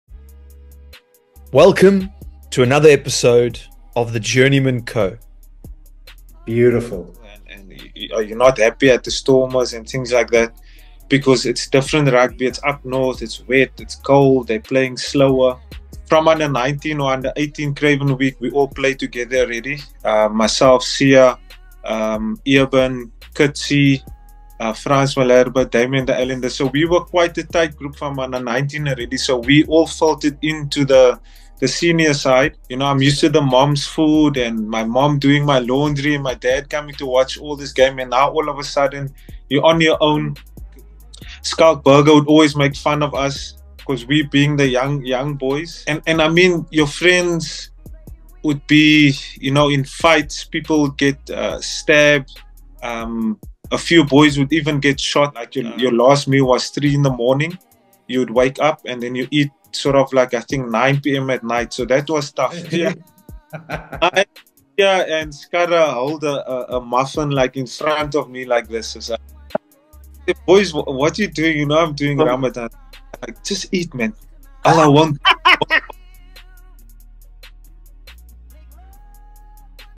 Episode 10 Preview - Nizaam Carr
Preview for EP 10 with Springbok and Blue Bulls flanker, Nizaam Carr.